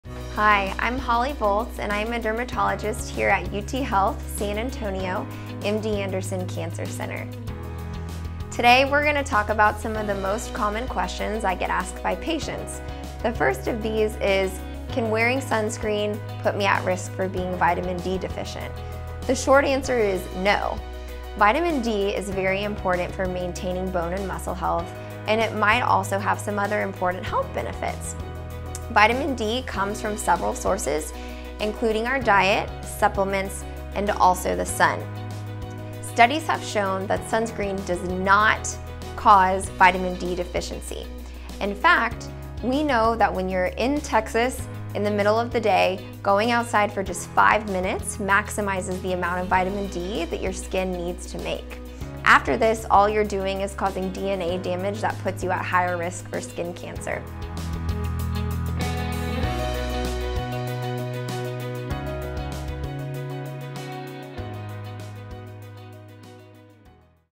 Does sunscreen cause Vitamin D deficiency? - Holly Volz, M.D.
Dr. Holly Volz, M.D., discusses the myth that sunscreen causes Vitamin D deficiency.